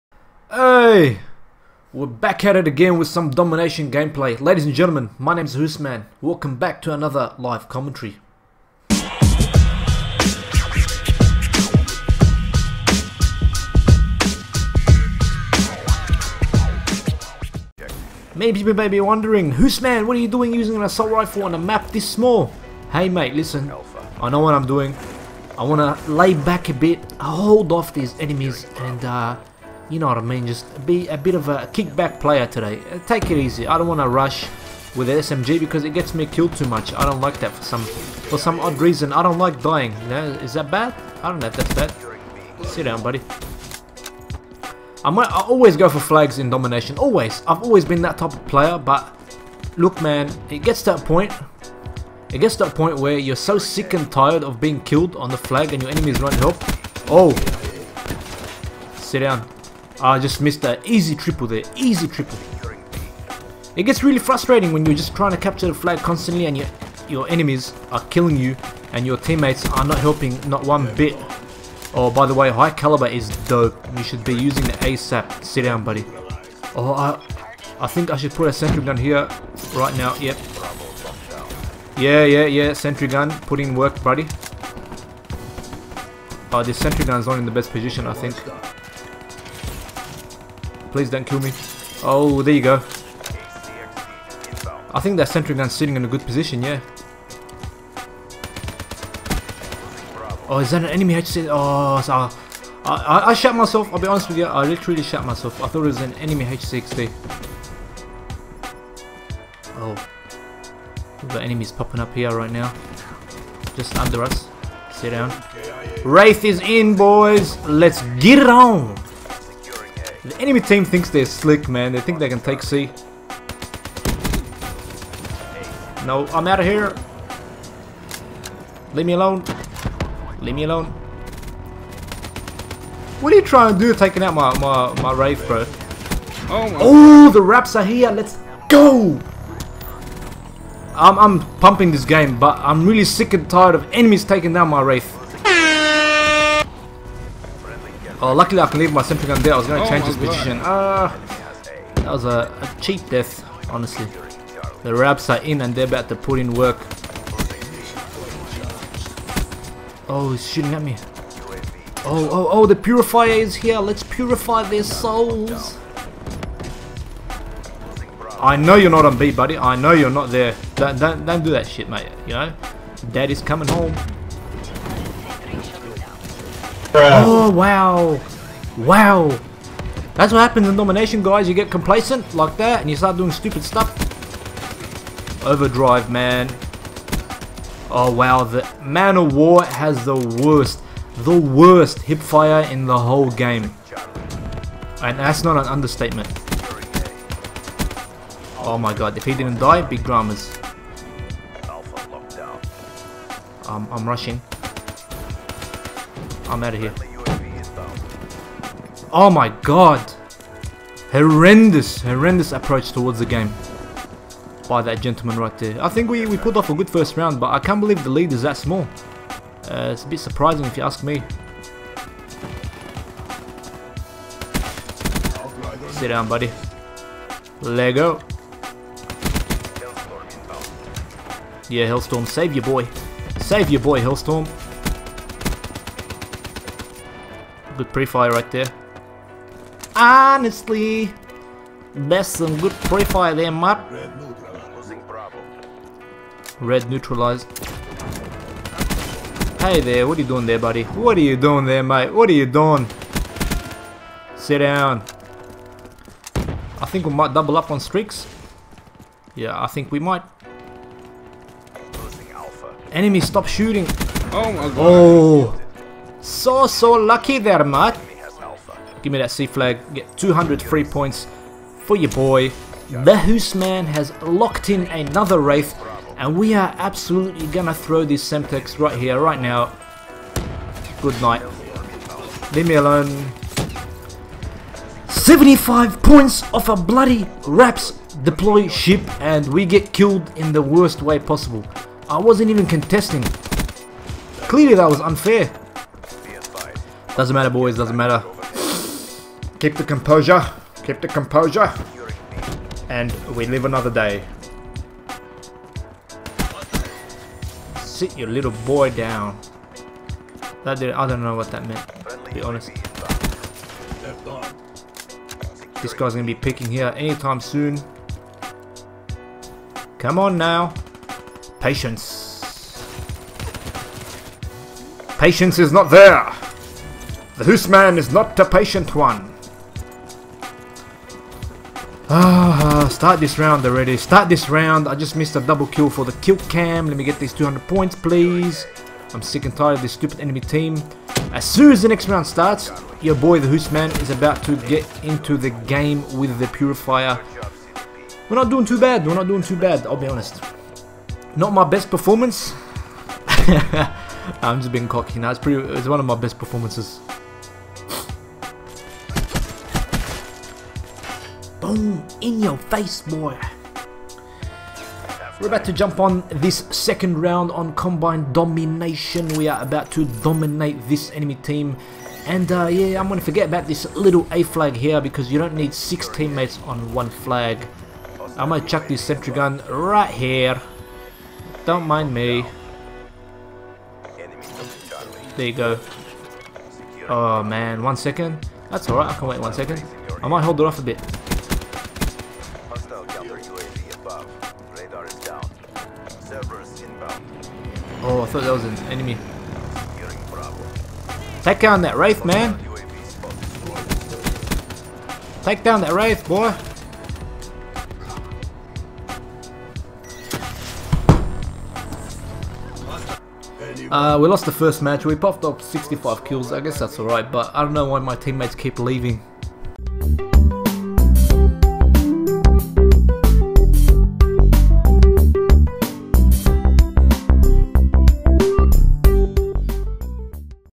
GET DOMINATED MATE!!! (60+ Kills Domination Live commentary)
I play heaps of domination in my own time but because of their crazy nature I never commentate. However, the time has come for me to commentate over a brutal beating of the enemy team in some domination.

Follow the Visage Team in their rise!

All gameplay recorded in 1080p60 with Elgato HD60.